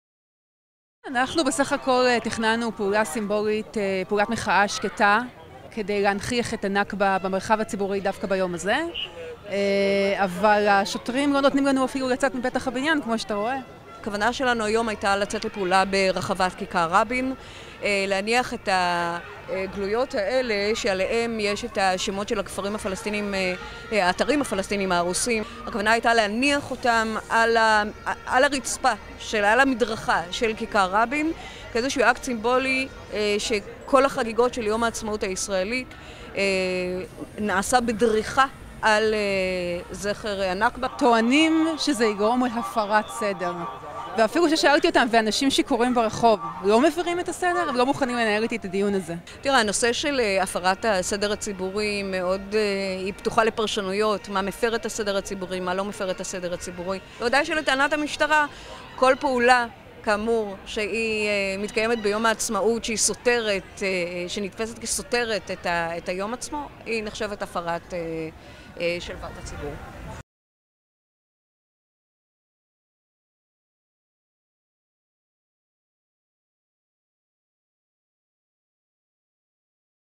כיתור זוכרות
פעילות ופעילים מעמותת זוכרות תכננו לעשות פעולת הסברה בכיכר רבין בתל אביב במהלך חגיגות העצמאות הישראלית. משטרת ישראל מנעה מהם לצאת ממשרדי העמותה וכתרה את המקום למשך שעות.